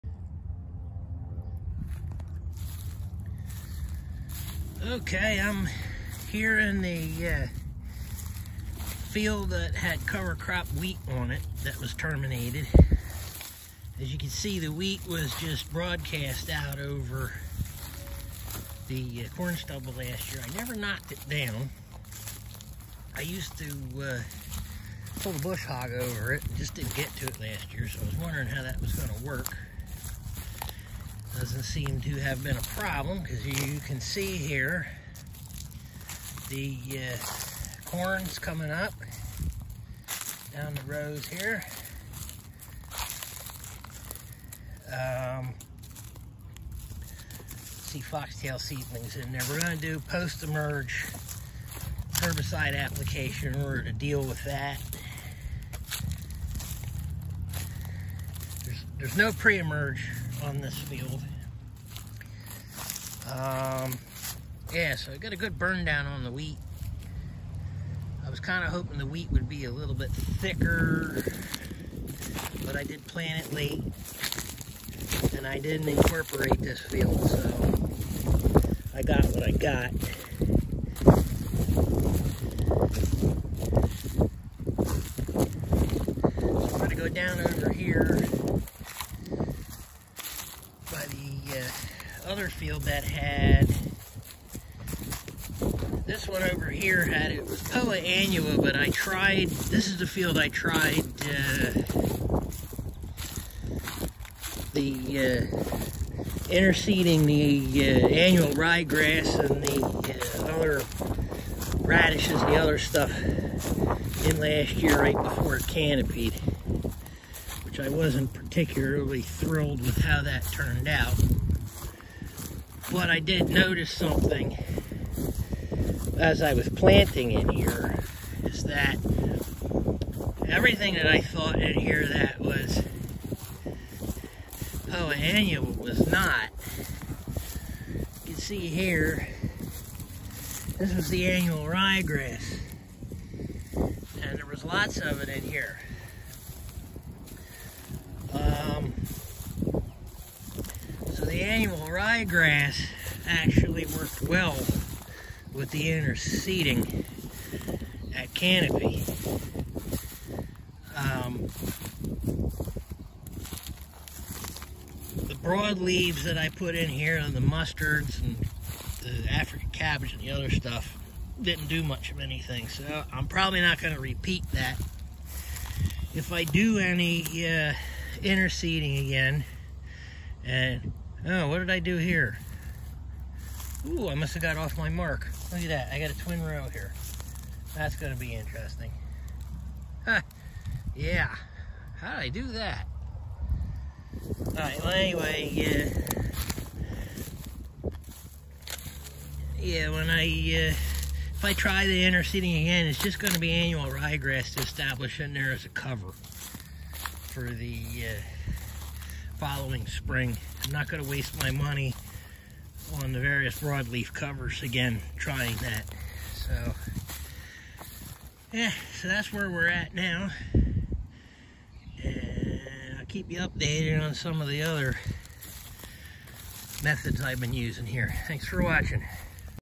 Corn coming up in cover crop wheat and surprised by the ryegrass.
This was the day before I was injured working on a hay rake.